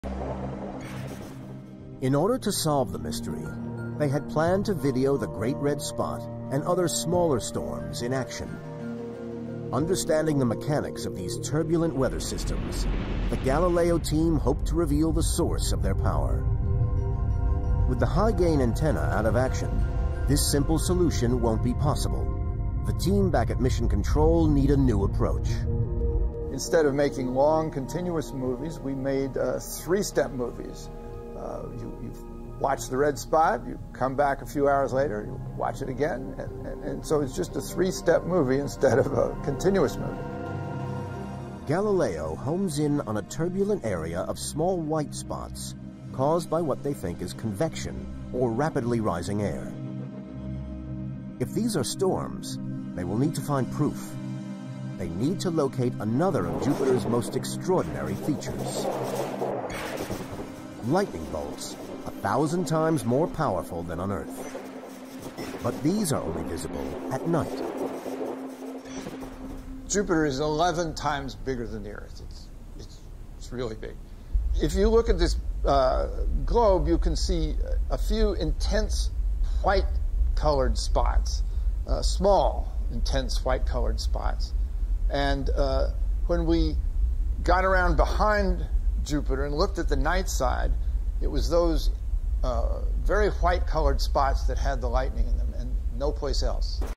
Dive into the electrifying storms of Jupiter! ⚡🌌
Witness the awe-inspiring power of Jupiter's weather systems as discoveries unfold! With the Galileo mission's innovative approach, scientists delve into the Great Red Spot and document the immense lightning phenomena on the night side of the planet. This adventure showcases the challenges faced by the Mission Control team and their resourcefulness in portraying these stunning storms with breathtaking clarity. How do these massive storms work? What secrets do they hold? Uncover the mysteries of our solar system’s largest planet, where lightning strikes a thousand times more potent than Earth’s illuminate the darkness!